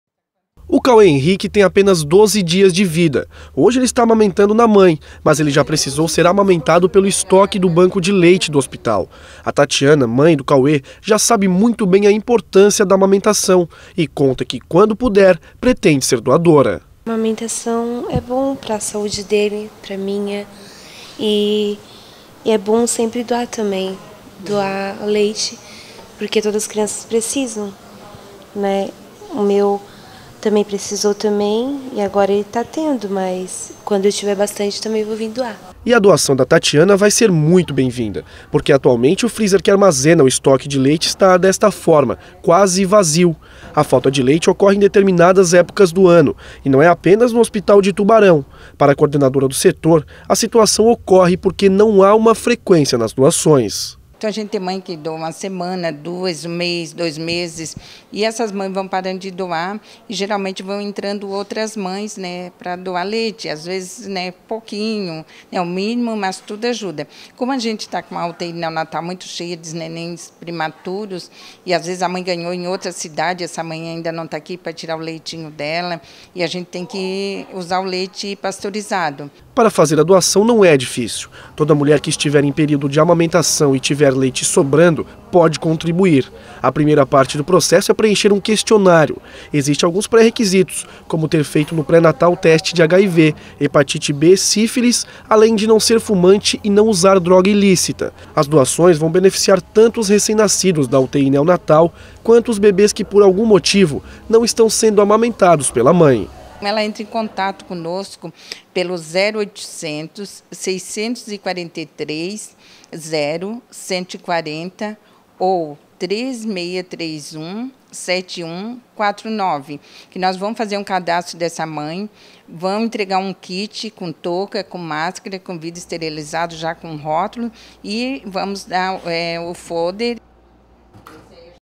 Banco de Leite do HNSC está com estoque baixo
O estoque do Banco de Leite do Hospital Nossa Senhora da Conceição em Tubarão está baixo. As doações beneficiam tanto os recém-nascidos da UTI Neonatal, quanto os bebês, que por algum motivo, não conseguem receber o leite da própria mãe